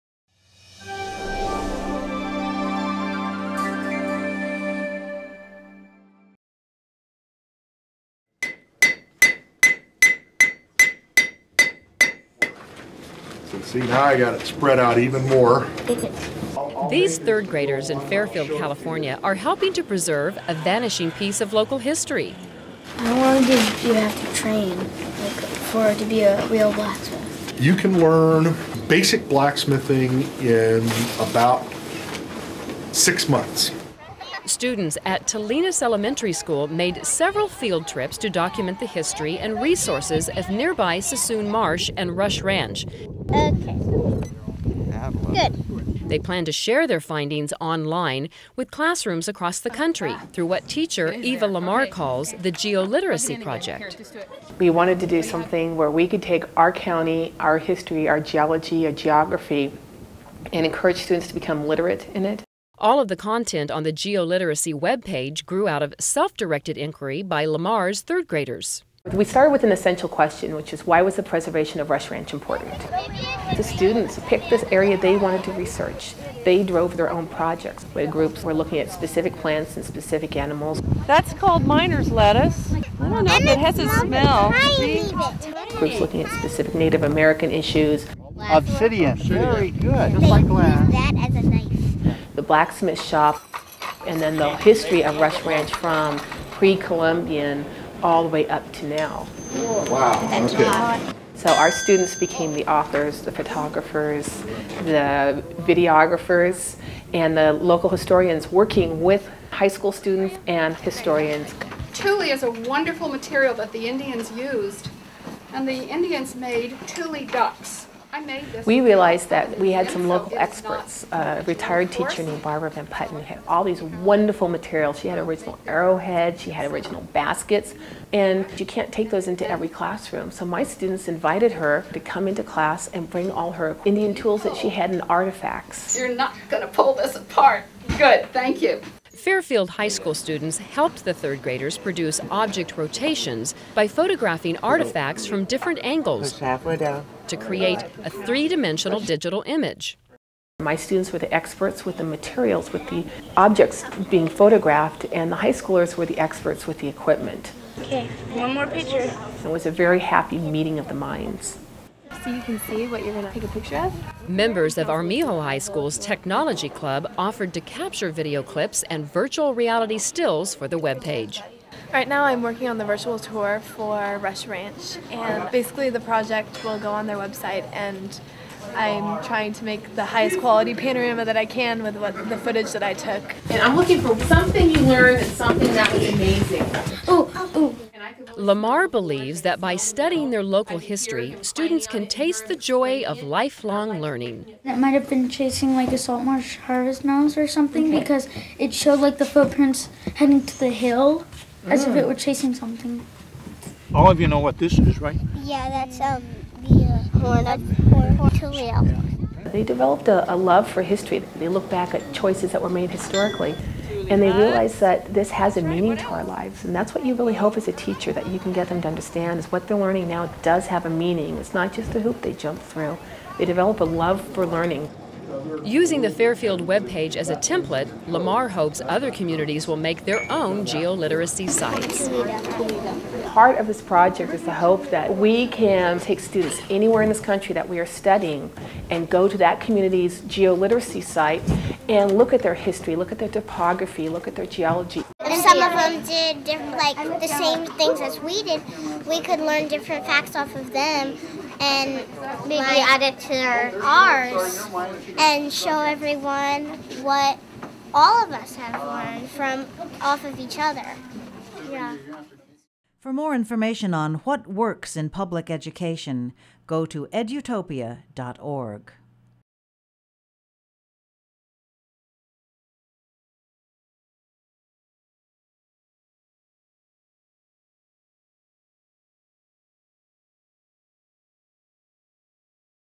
Using Technology to Discover Geography and Geology Outside the Classroom
Third graders become historians, writers, and videographers as they explore the geography and geology of their community.